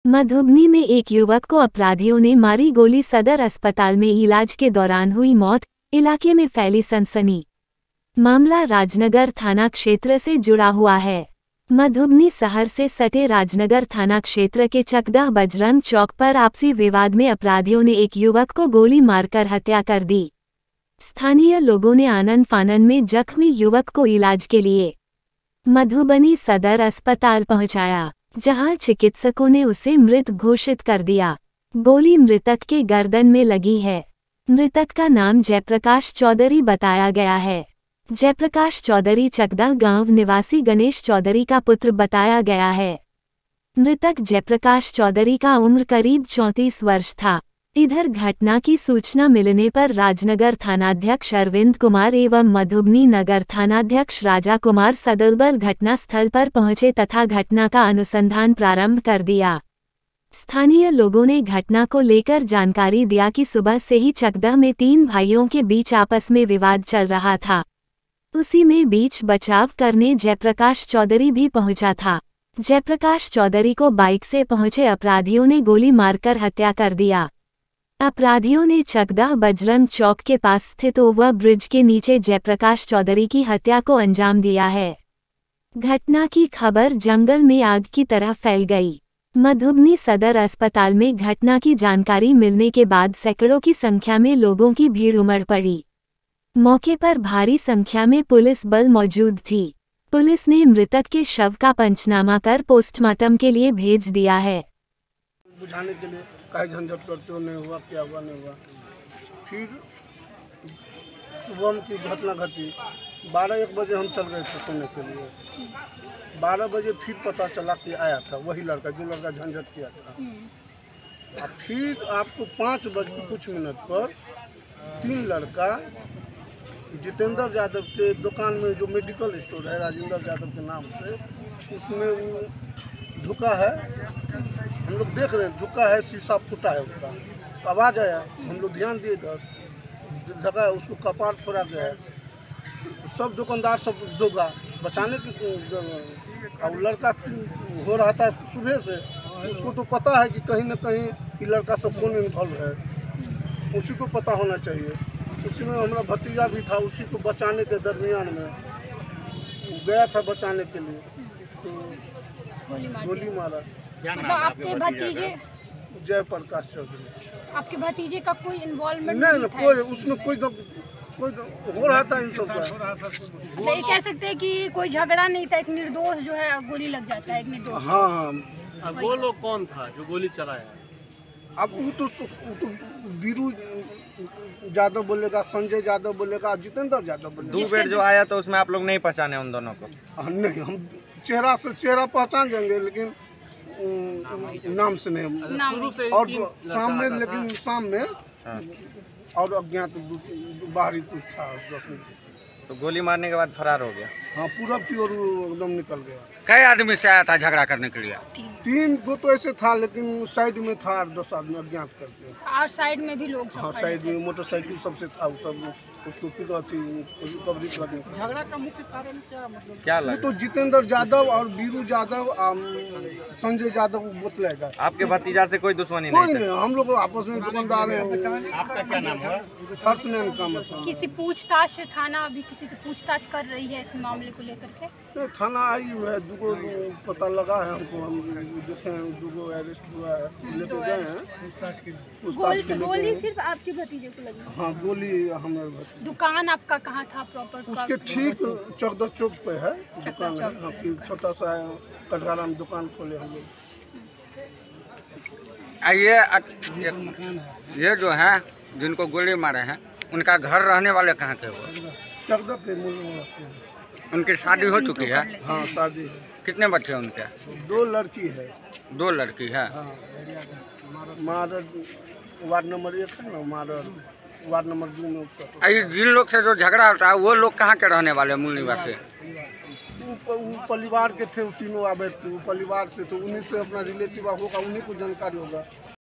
मधुबनीं में दिन-दहाड़े एक व्यक्ति को मारी गोली,हुई मौत..पहुंची पुलिस जांच सुरु
मधुबनीं में एक युवक को अपराधियों नें मारी गोली सदर अस्पताल में ईलाज के दौरान हुई मौत,इलाके में फैली सनसनी।
मामला राजनगर थाना क्षेत्र से जुड़ा हुआ है।
मधुबनीं सहर से सटे राजनगर थाना क्षेत्र के चकदह बजरंग चौक पर आपसी विवाद में अपराधियों ने एक युवक को गोली मारकर हत्या कर दी
स्थानीय लोगों ने आनन-फानन में जख्मी युवक को ईलाज के लिए मधुबनी सदर अस्पताल पहुंचाया। जहां चिकित्सकों ने उसे मृत घोषित कर दिया।
गोली मृतक के गर्दन में लगी है।
मृतक का नाम जयप्रकाश चौधरी बताया गया है। जयप्रकाश चौधरी चकदह गांव निवासी गणेश चौधरी का पुत्र बताया गया है। मृतक जयप्रकाश चौधरी का उम्र करीब 34 वर्ष था।
इधर घटना की सूचना मिलनें पर राजनगर थानाध्यक्ष अरविंद कुमार एवं मधुबनीं नगर थानाध्यक्ष राजा कुमार सदलबल घटना स्थल पर पहुंचे तथा घटना का अनुसंधान प्रारंभ कर दिया।स्थानीय लोगों ने घटना को लेकर जानकारी दिया कि सुबह से ही चकदह में तीन भाइयों के बीच आपस मे विवाद चल रहा था। उसी में बीच बचाव करनें जयप्रकाश चौधरी भी पहुंचा था। जयप्रकाश चौधरी को बाईक से पहुंचे अपराधियों ने गोली मारकर हत्या कर दिया। अपराधियों नें चकदह बजरंग चौक के पास स्थित ओवर ब्रिज के नीचे जयप्रकाश चौधरी की हत्या को अंजाम दीया है। घटना की खबर जंगल मे आग की तरह फैल गई। मधुबनीं सदर अस्पताल में घटना की जानकारी मिलने के बाद सैकड़ो की संख्या में लोगों की भीड़ उमड़ पड़ी। मौके पर भारी संख्या में पुलिस बल मौजूद थी। पुलिस नें मृतक के शव का पंचनामा कर पोस्टमार्टम के लिए भेज दिया है।